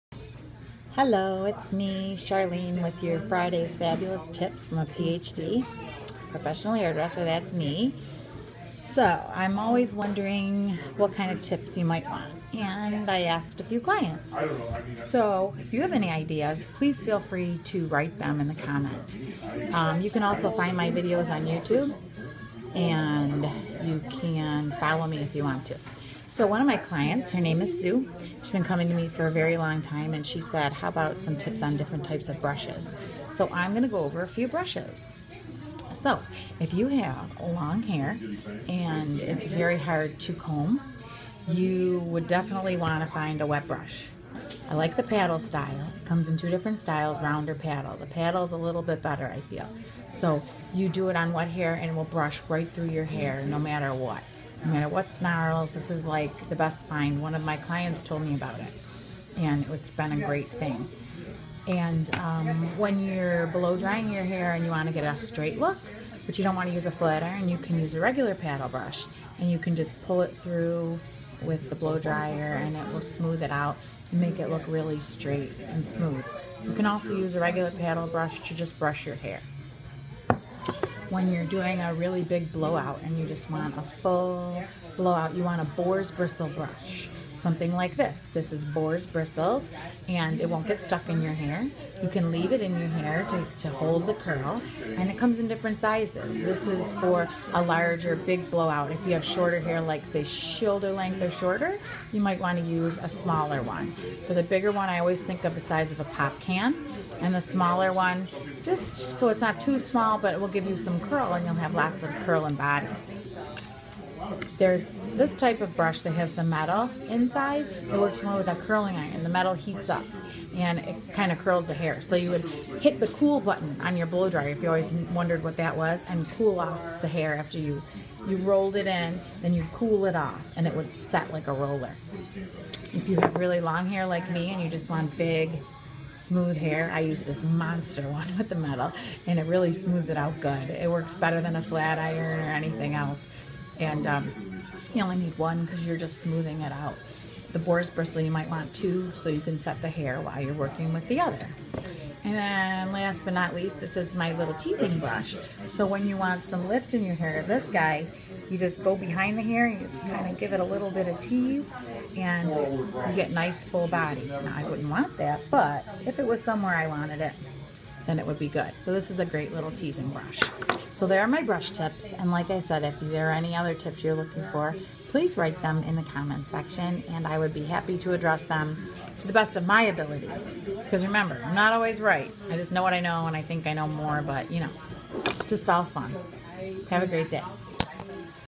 Fridays fabulous tips
Which brush is right for you? Here a few ways to use a few types of brushes.